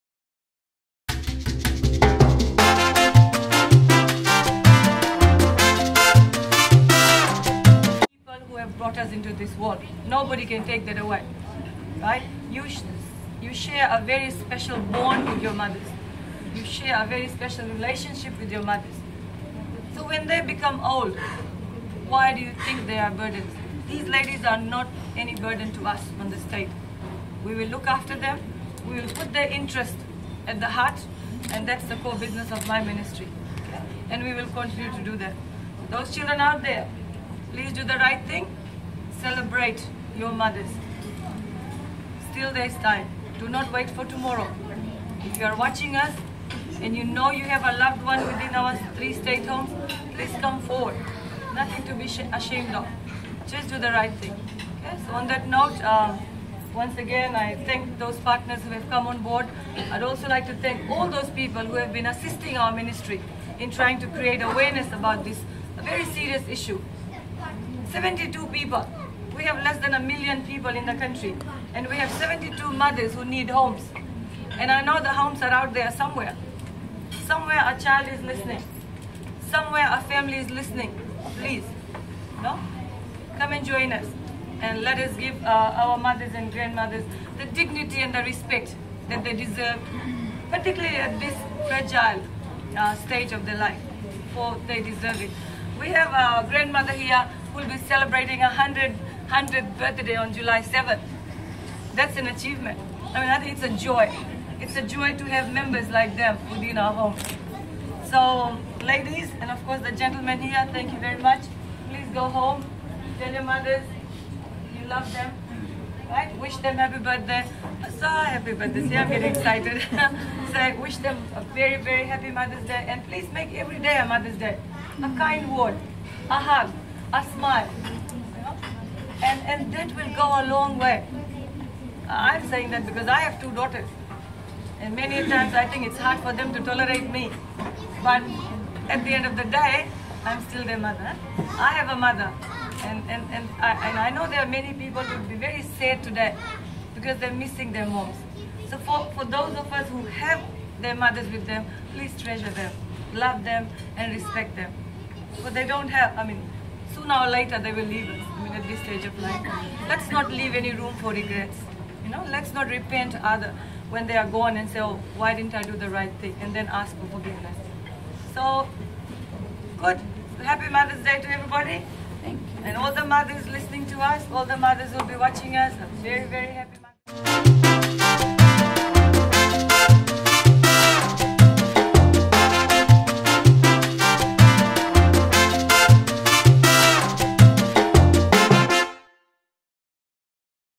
Minister for Women, Children and Poverty Alleviation , Rosy Akbar’s Mothers Day Message
Minister for Women, Children and Poverty Alleviation , Rosy Akbar’s Mothers Day Message during celebrations at the New Perking Restaurant today with Samabula Old People's Home residents